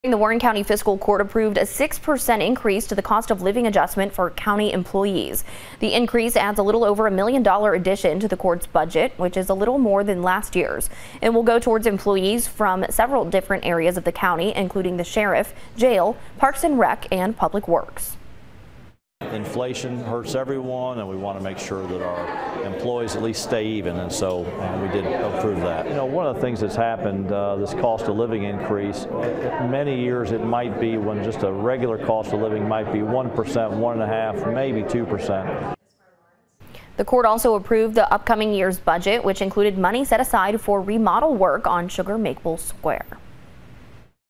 Warren County Fiscal Court approves cost of living adjustment for county employees
The Warren County Fiscal Court meeting has approved a 6% increase to the cost of living adjustment for county employees.